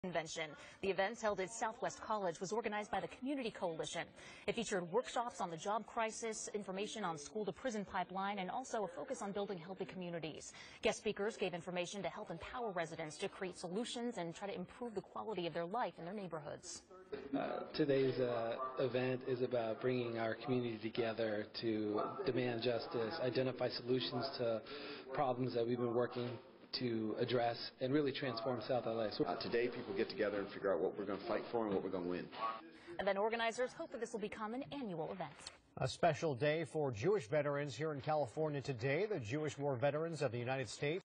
First Annual People Power Convention
In June 2016, over 400 people attended a community conference at Southwest College in South L.A. Residents to talked about what they experience in our community, and received training by experts on topics of health, civic engagement, mass incarceration and the economic crisis.